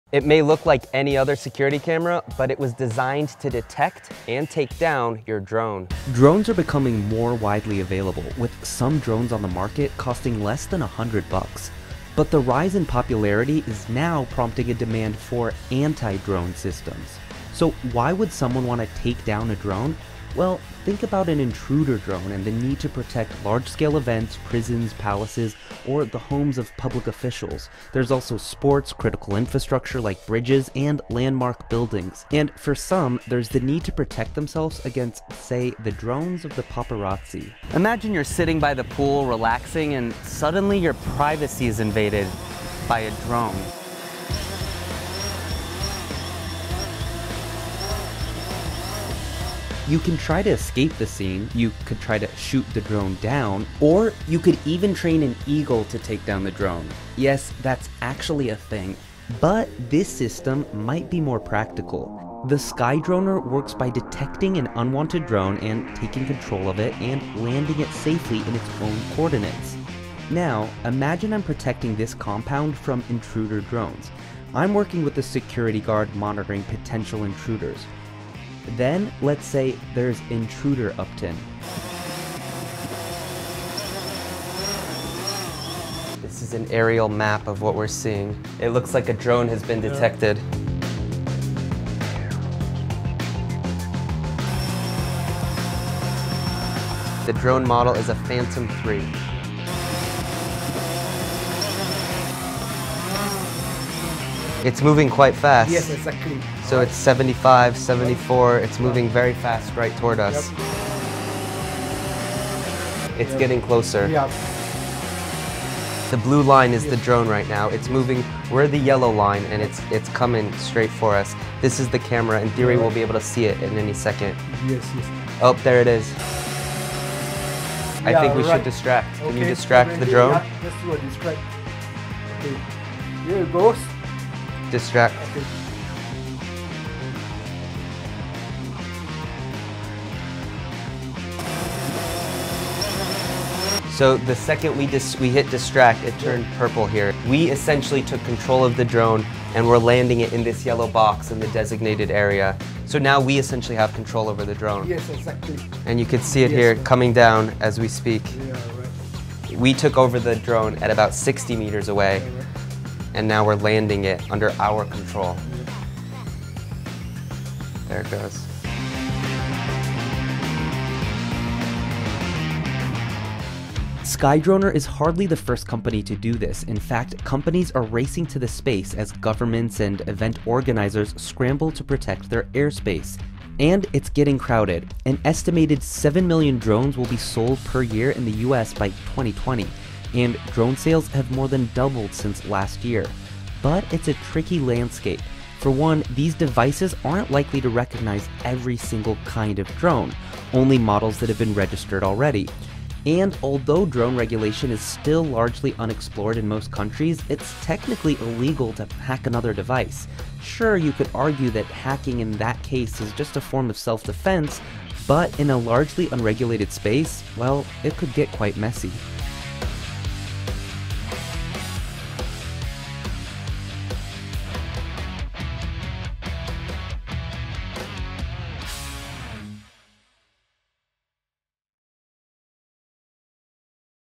This camera detects, hacks and takes down drones | CNBC Reports
As drones become more popular so does the need for protection against the devices. CNBC's Uptin Saiidi tests out new anti-drone technology.